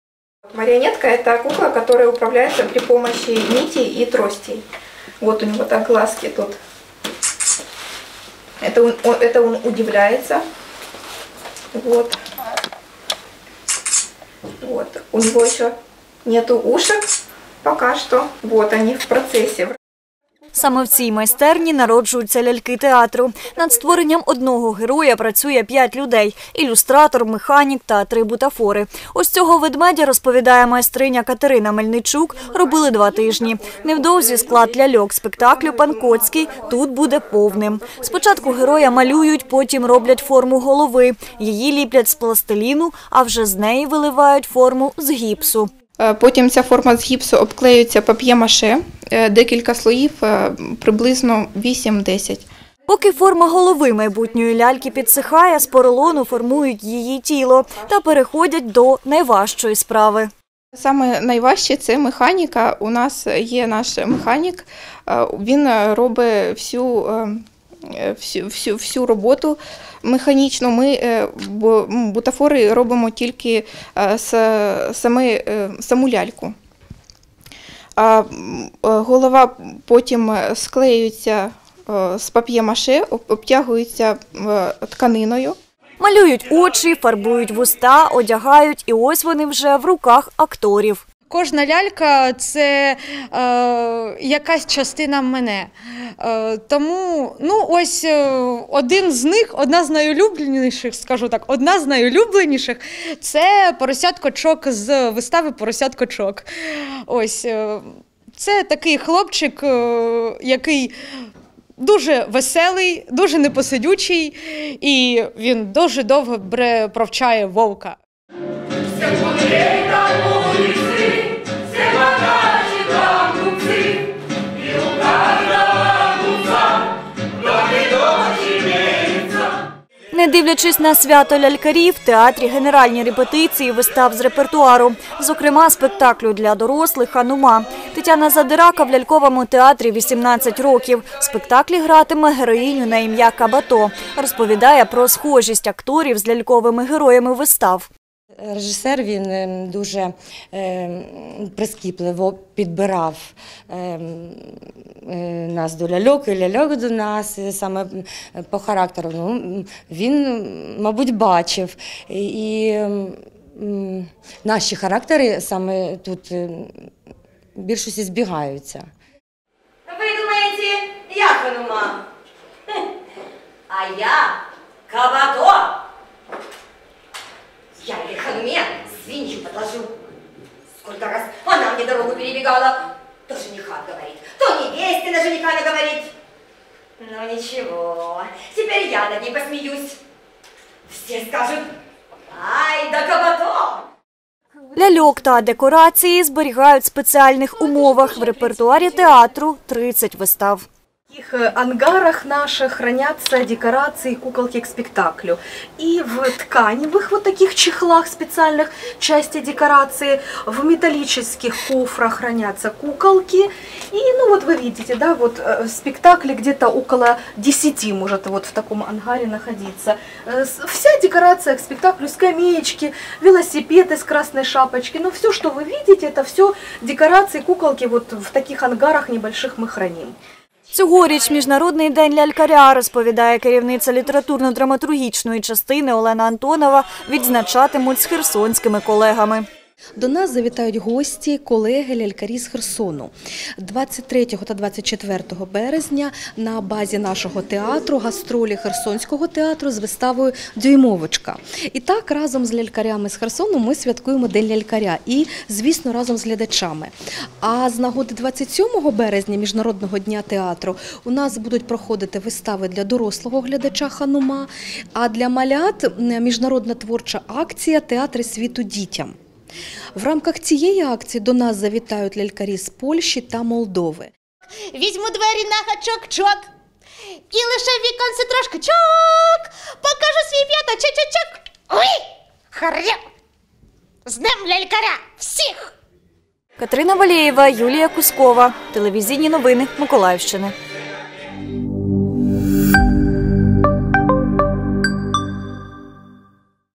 У театрі ляльок тривають репетиції вистави для дорослих
21 березня в  Миколаївському академічному обласному театрі ляльок тривають генеральні репетиції вистави для дорослих «Ханума». За лаштунками театру, а також у майстерні, де народжуються ляльки, у Міжнародний день лялькаря побували кореспонденти Суспільного.